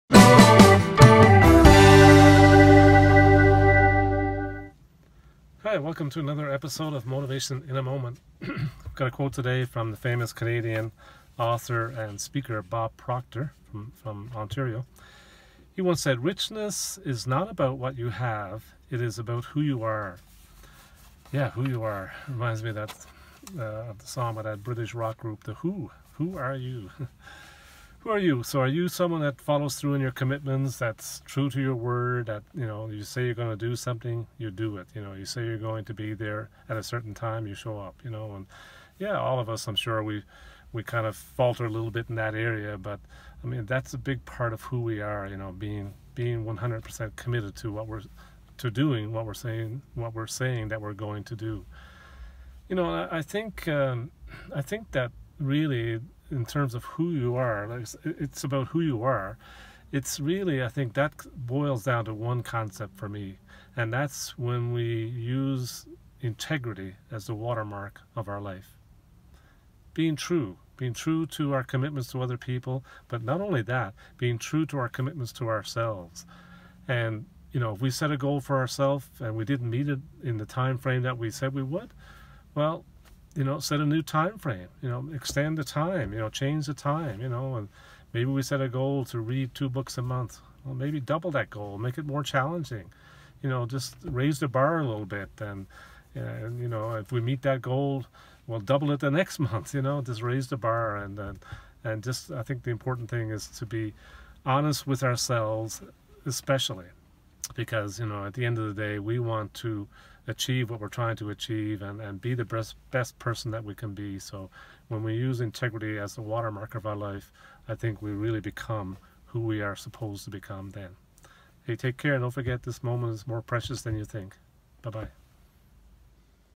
Motivation in a Moment- 70. “The Watermark of Integrity”
In this video, I talk about how we can truly define our life.